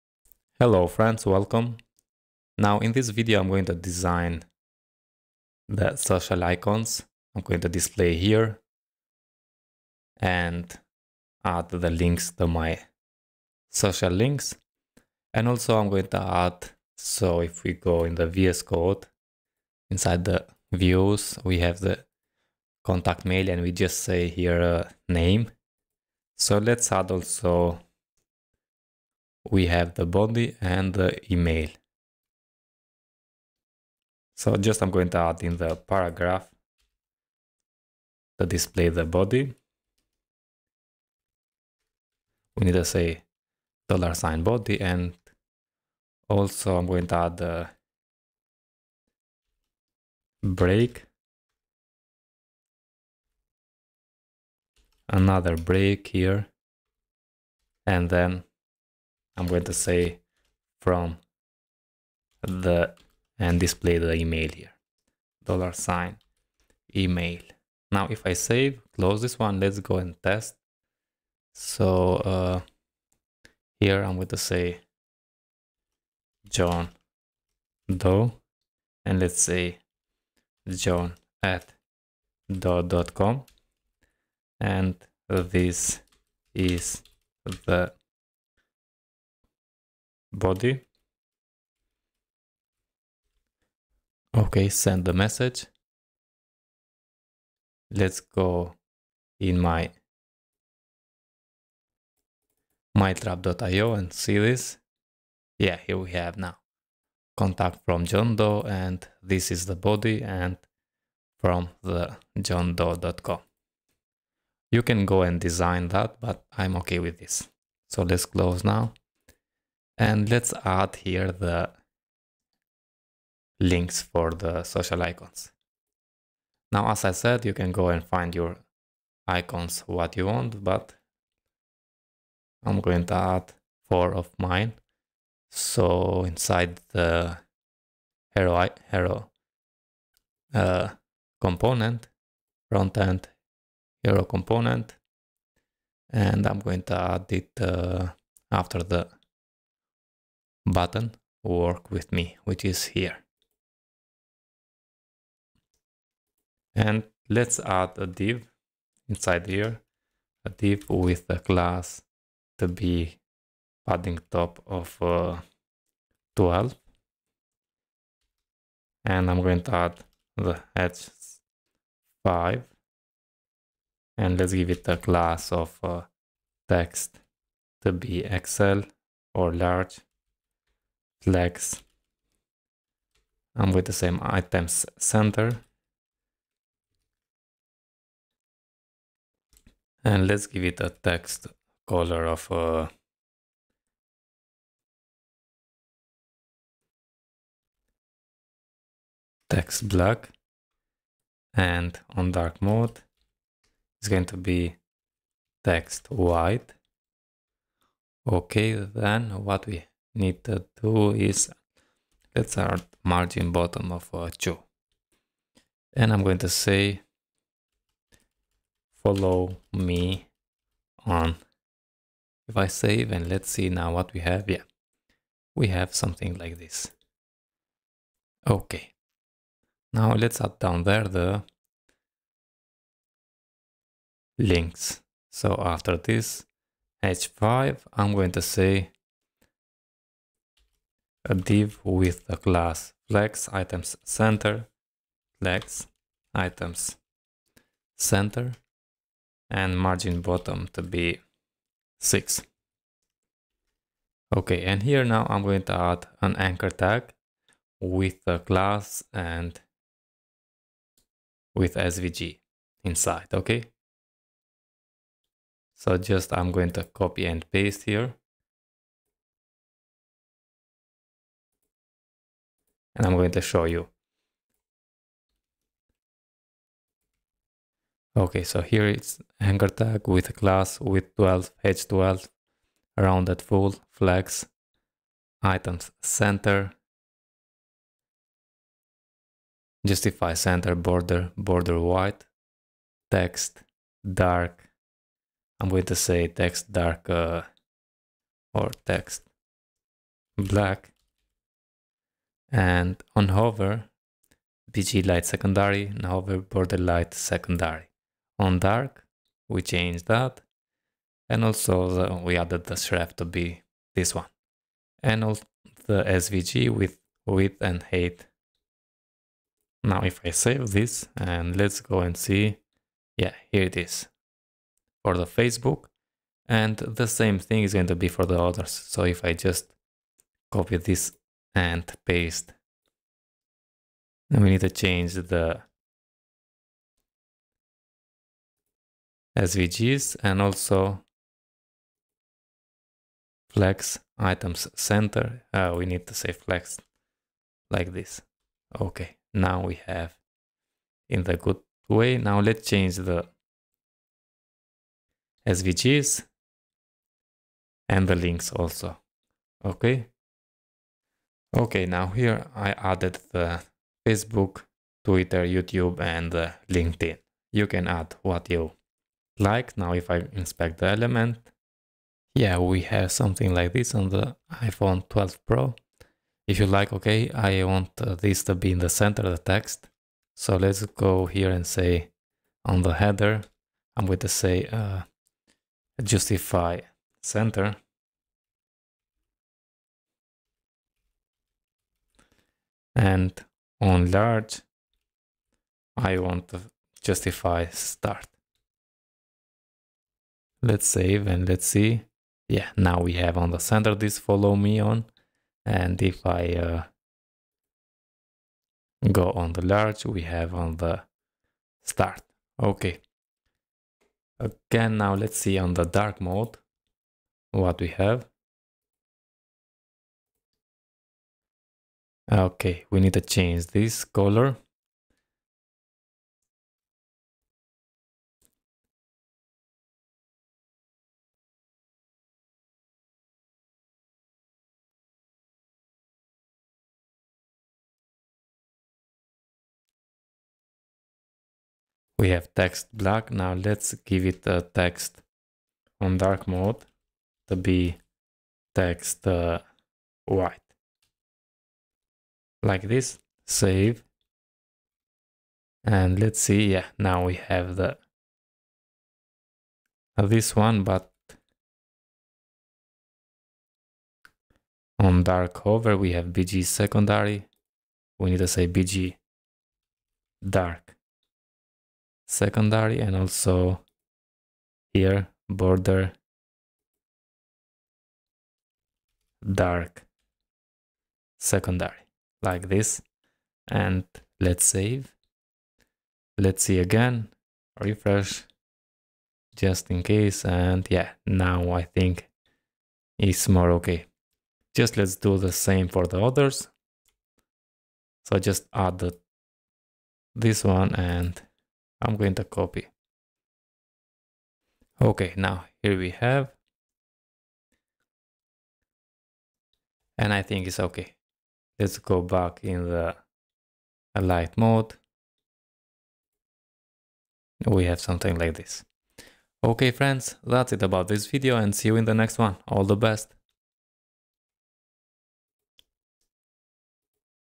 26 Add Social Icons | Portfolio Project with Laravel and Vuejs
This video will show you how to design social icons.
Learn how to build a Portfolio project with Laravel Inertiajs and Vuejs.
Laravel Portfolio project.
VueJS portfolio project.
Laravel tutorial.
Laravel vuejs tutorial.
Laravel inertia tutorial.